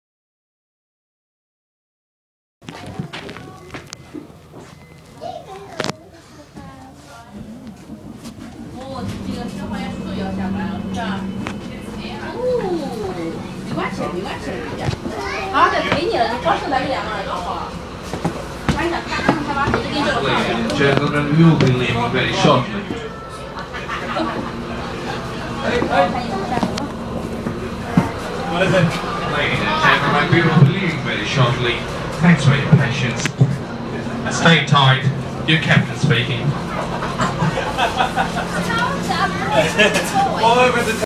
CDC Victoria's New Double Deck Bus
Video of the inside of CDC Victoria's New double-deck bus,